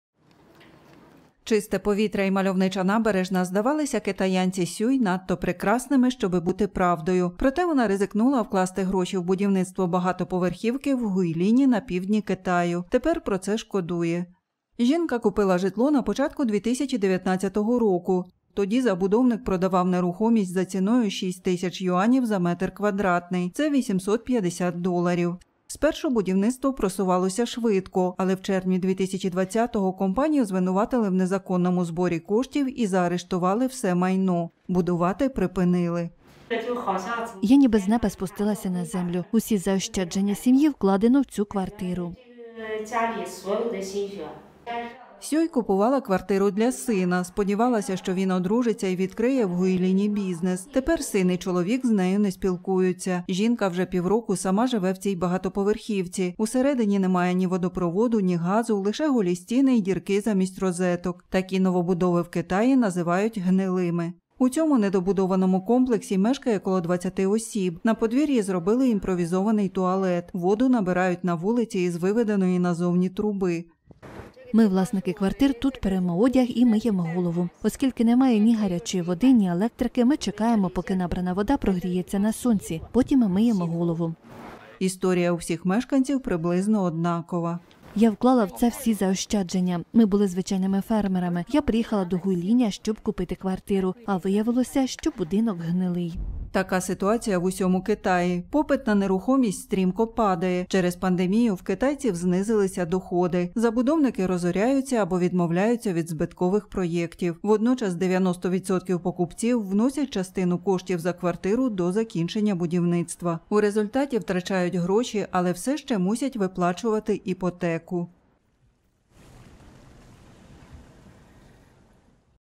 Покинуті гнити багатоповерхівки: тисячі китайців постраждали через кризу на ринку нерухомості
Чисте повітря й мальовнича набережна здавалися китаянці Сюй надто прекрасними, щоби бути правдою. Проте вона ризикнула вкласти гроші в будівництво багатоповерхівки в Гуйліні на півдні Китаю. Тепер про це шкодує.